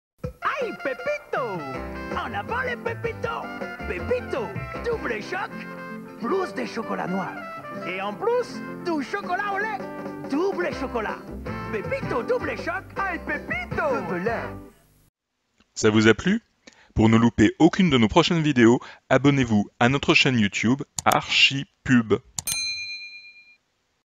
Publicité Pepito Double-Choc - 1994
Spot de pub Pepito Double-Choc
Annonceur : Belin - Groupe Danone
Agence : FCA
France - 1994